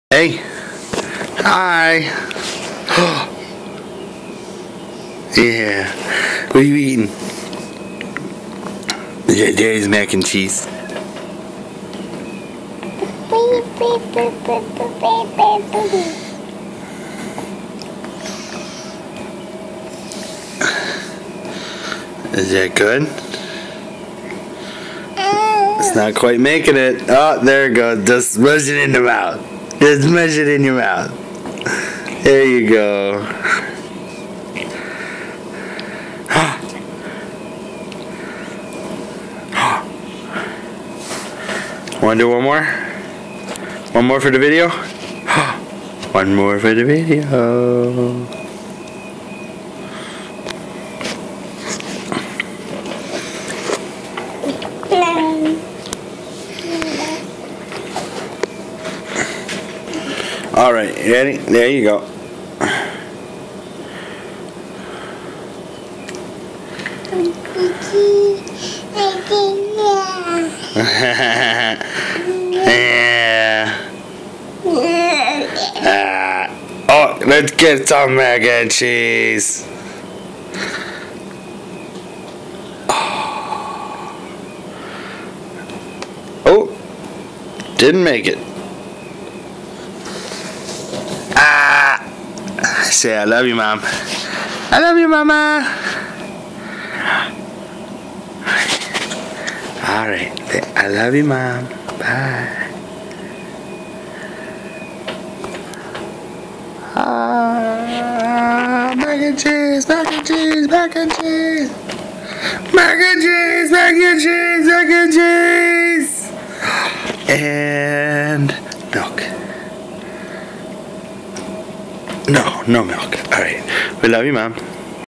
Dommy loves mac & cheese
I made box mac & cheese with cheddar, smoked guda and salami he loved it.